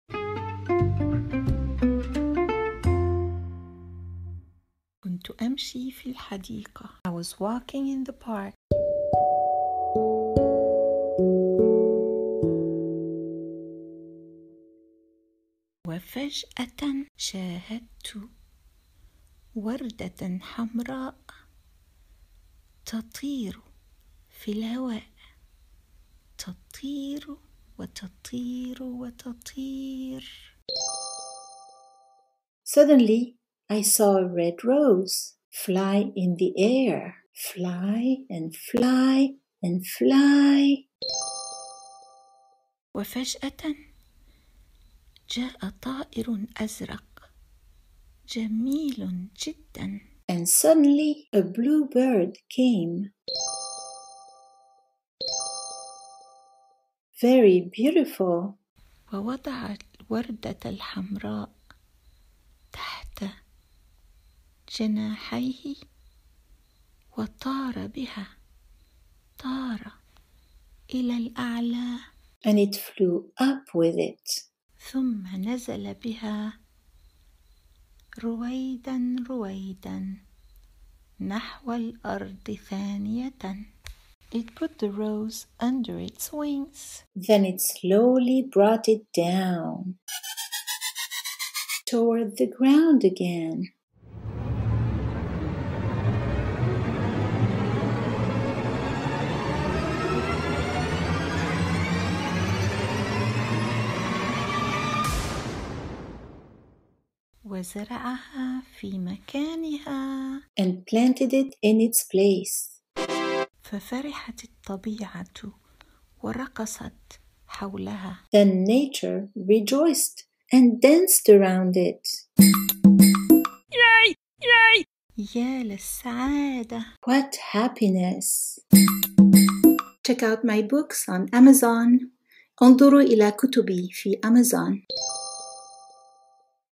Bilingual (Ar-En) Kids' Story : Blue Bird Saves the Red Flower ‏الطائر الازرق ينقذ الوردة الحمراء
Blue Bird Saves Red Flower- Arabic Version ‏الطائر الازرق ينقذ الوردة الحمراء

1. كنت أمشي في الحديقة

2. وفجأة... شاهدت وردة حمراء

3. تطير في الهواء

4. تطير وتطير وتطير

5. وفجأة

6. جاء طائر أزرق

7. جميل جدا

8. ووضع الوردة الحمراء تحت جناحيه

9. وطار بها إلى الأعلى

10. ثم نزل بها رويدا رويدا

11. نحو الأرض ثانية

12. وزرعها في مكانها

13. ففرحت الطبيعة ورقصت حولها

14. يا للسعادة!

1. I was walking in the park

2. Suddenly ... I saw a red rose

3. Fly in the air

4. Fly and fly and fly

5. And suddenly

6. A blue bird came

7. Very beautiful

8. It placed a red rose under its wings

9. It flew it up

10. Then it slowly brought it down

11. Toward the ground again

12. And planted it in its place

13. Then nature rejoiced and danced around it

14.What happiness!